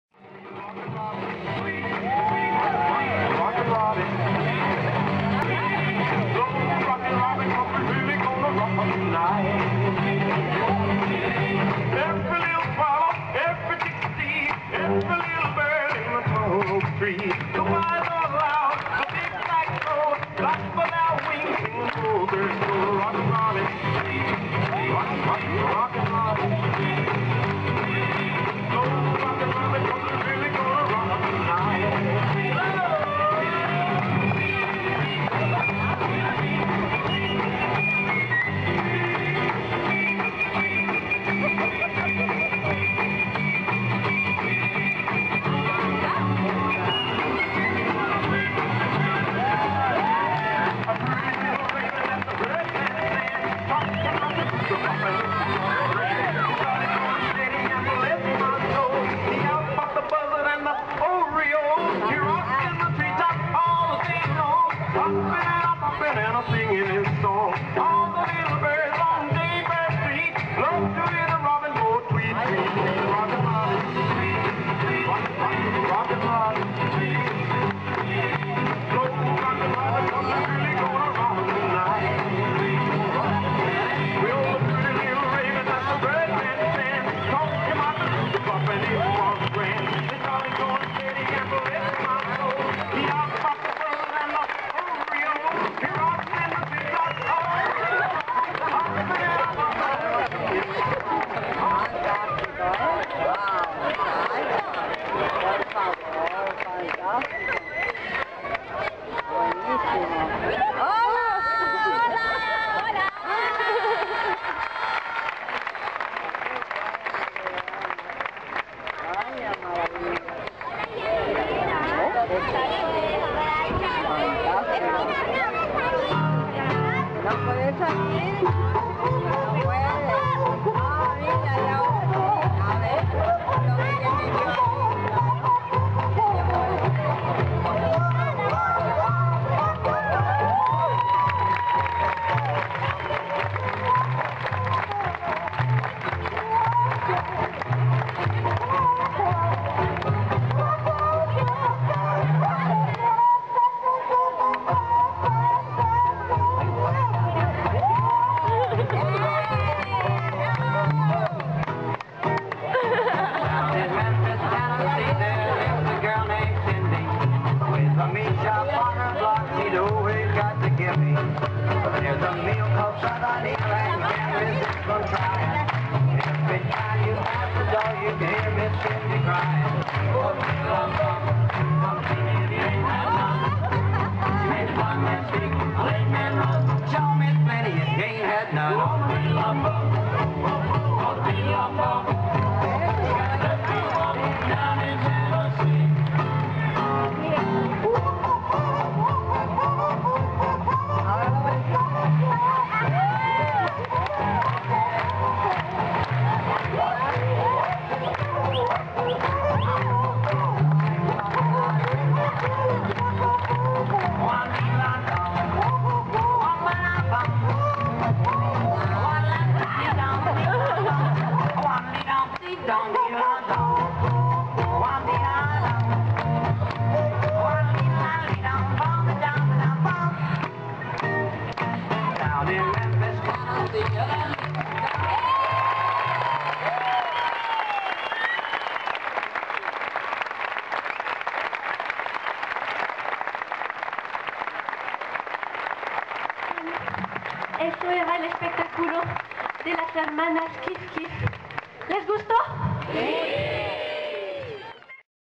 Emplume [The Kif-Kif sisters]
Parte de uno de los espectáculos del Mueca 2012 [Puerto de la Cruz - Tenerife]